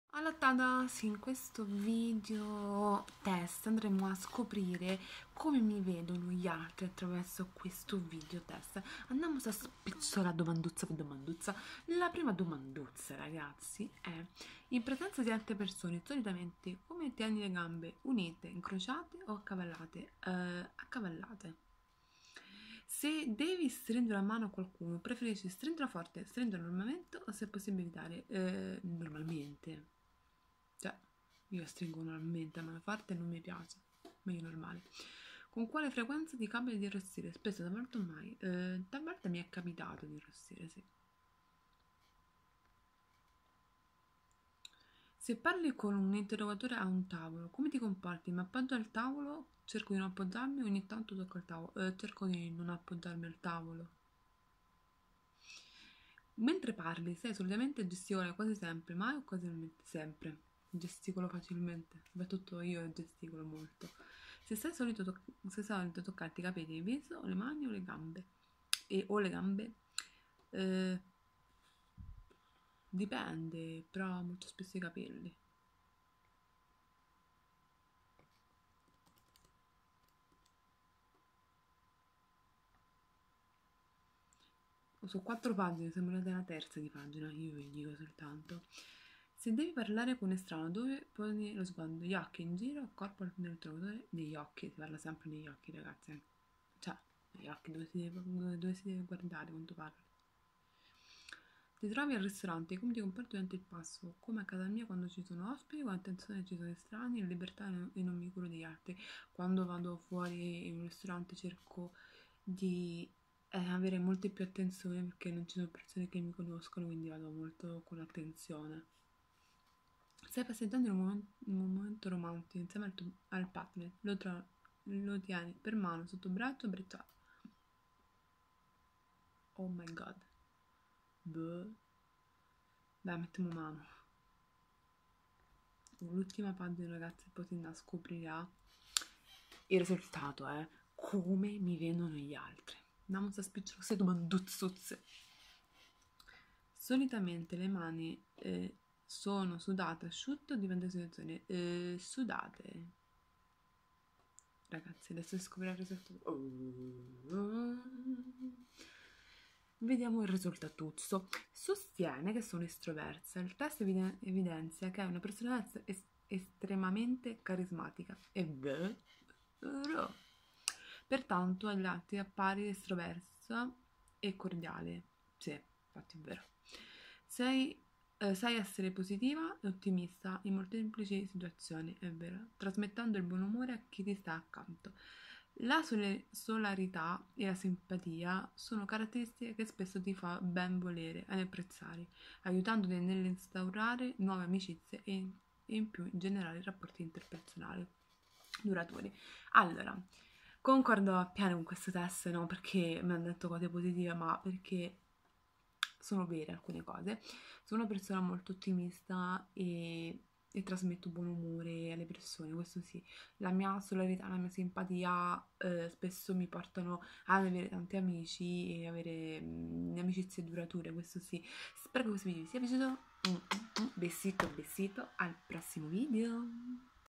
COME MI VEDONO GLI ALTRI??
Questo test scoprirete come gli altri mi vedono . Spero che questo test vi piacerà .